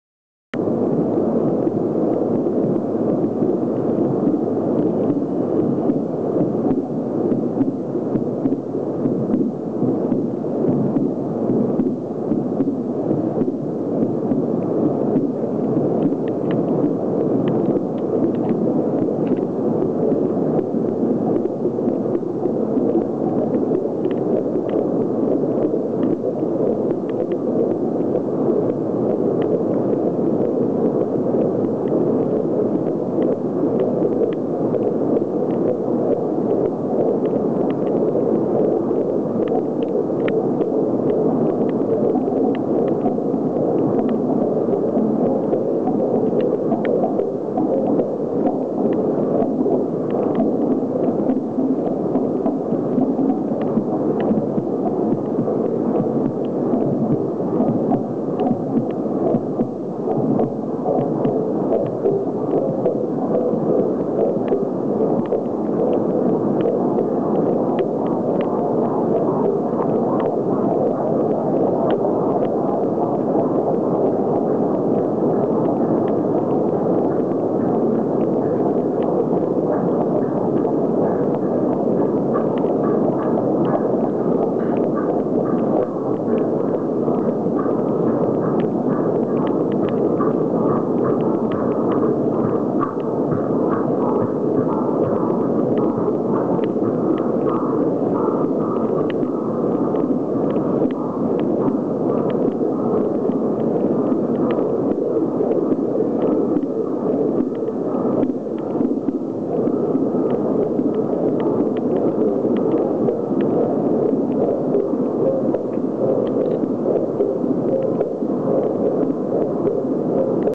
Monumenta: Boltanski "Personnes" 19/01/2010
Promenade dans Monumenta: installation de Christian Boltanski: "Personnes" 19/01/2010